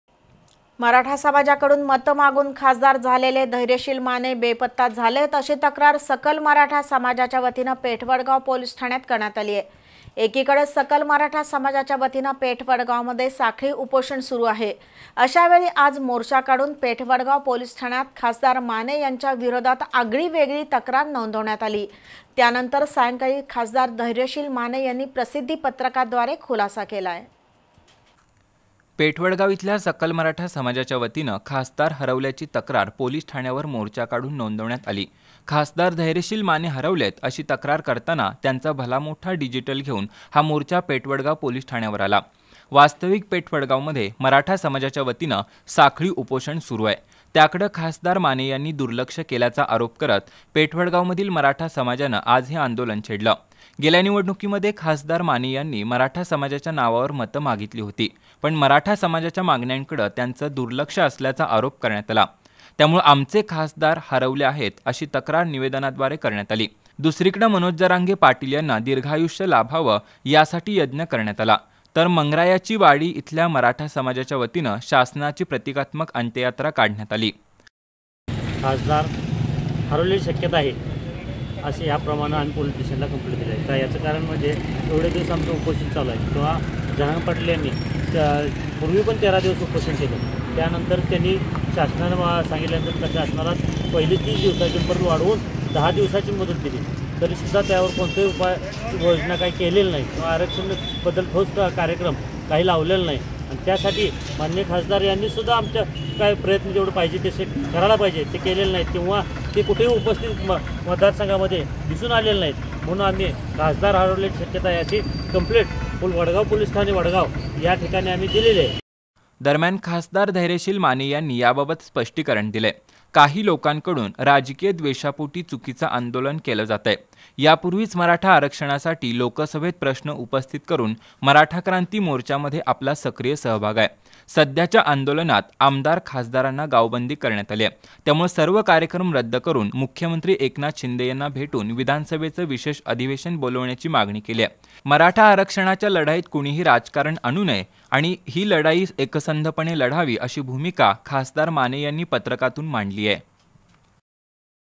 सकल मराठा समाजाच्यावतीनं मोर्चा काढून खासदार धैर्यशिल माने हरवल्याची पोलिस ठाण्यात झाली तक्रार
पेठवडगावमधील सकल मराठा समाजाच्यावतीनं मोर्चा काढून खासदार धैर्यशिल माने हरवल्याची पोलिस ठाण्यात झाली तक्रार, मंगरायाचीवाडी परिसरात निघाली शासनाची प्रतिकात्मक अंत्ययात्रा